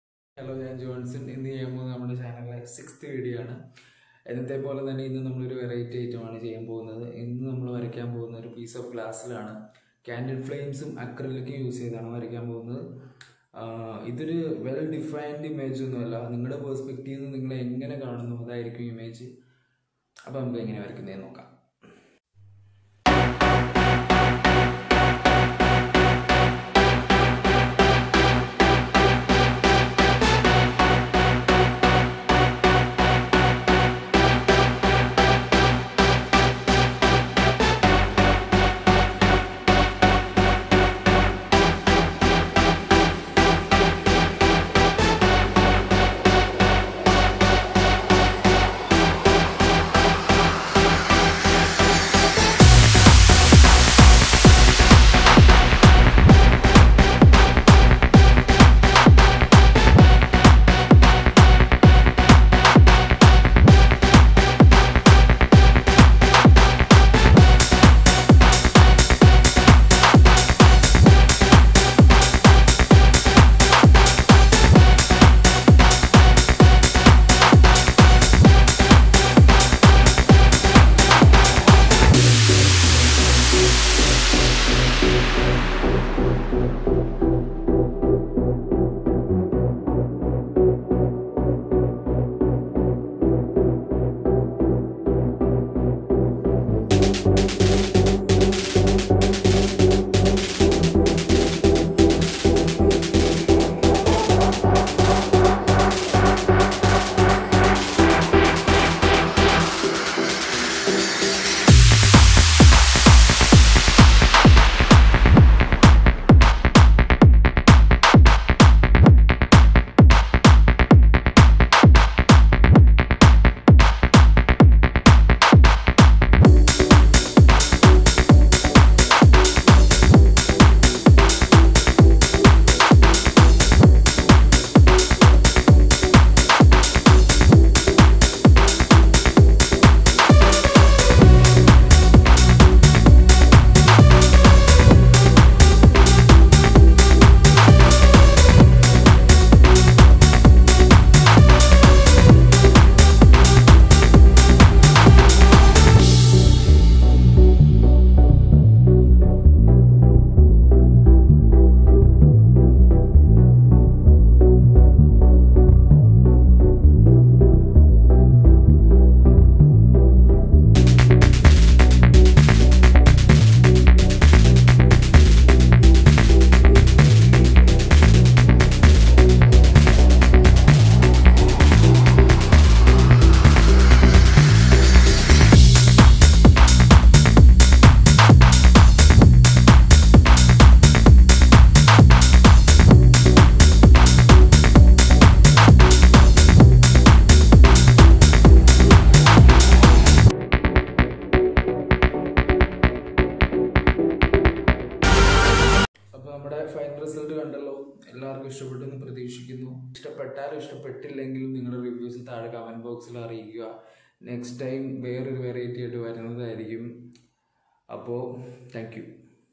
Magic of fire flames with acrylic painting / Acrylic Painting Tutorial techniques for beginners
Things used :
piece of glass
acrylic colour : white & black only
candle / lighter